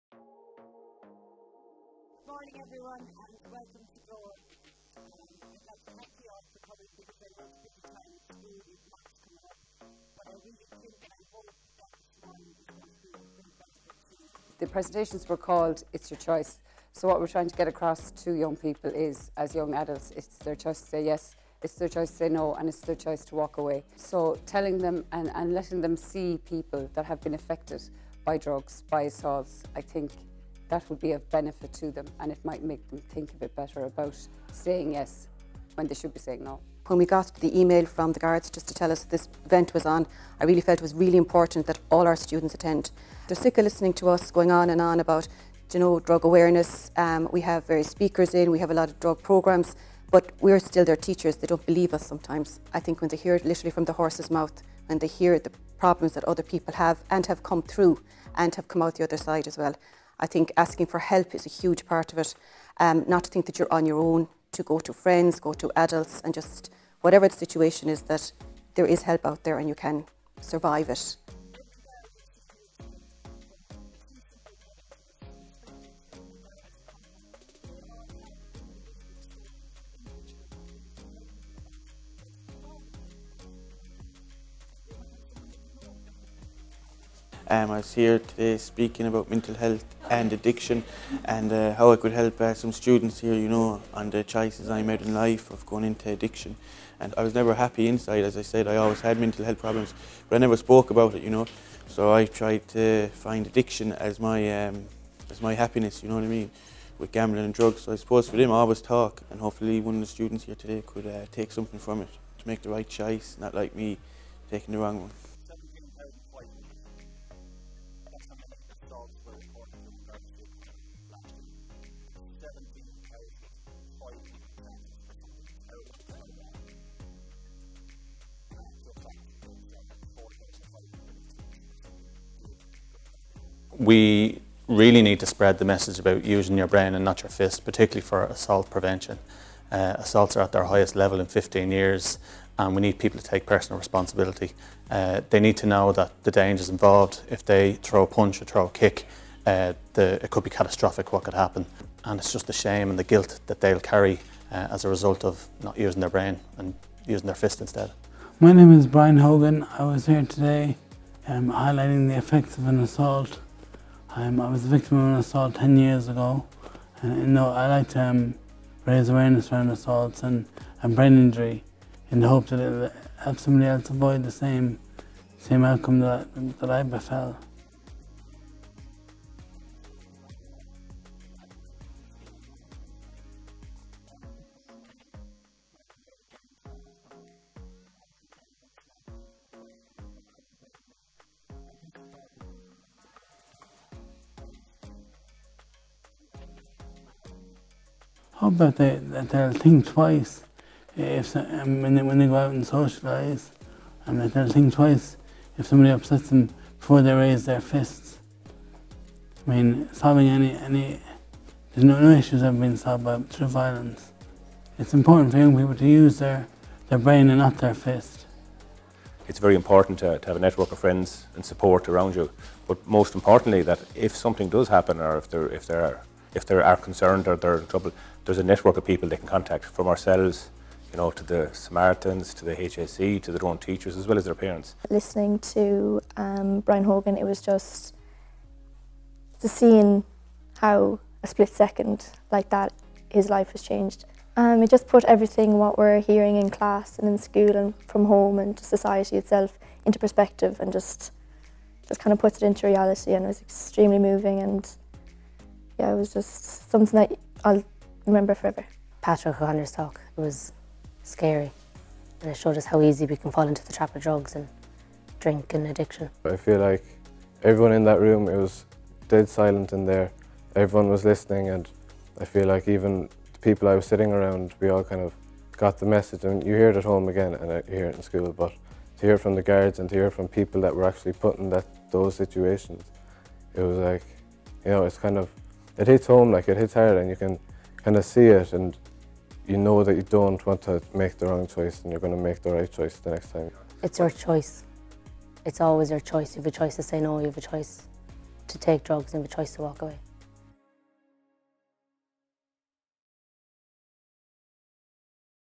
It's Your Choice - An Garda Síochána
Over 500 Leaving Certificate students attended the ‘It’s Your Choice’ event on 22 January 2020 in Ennis, Co. Clare. Students heard from a reformed drug/gambling addict, a victim of serious assault, and Gardaí on the subjects of sexual coercion, and the Use Your Brain Not Your Fist assault reduction campaign.